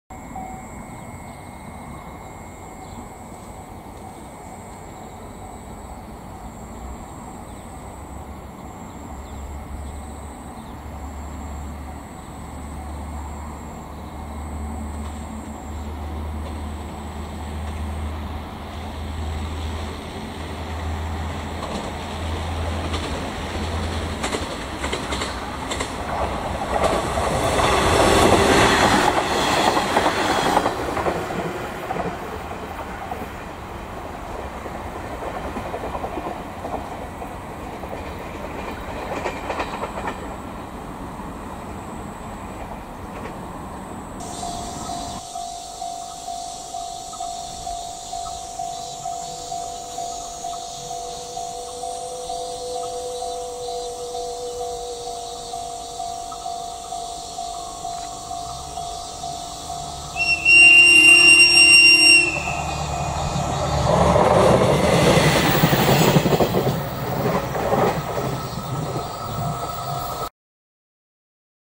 【キハ100単独】快速湯けむり号&キハ100-10 KY出場
撮影 2020/08/16 2020/08/19
快速湯けむり号
先月から運行開始した湯けむり号。
リゾートみのりと同じ時間帯で運行しています。
そして、盛ﾓﾘのキハ100-10が単独でKY出場しました。1両で本線出場はかわいいです。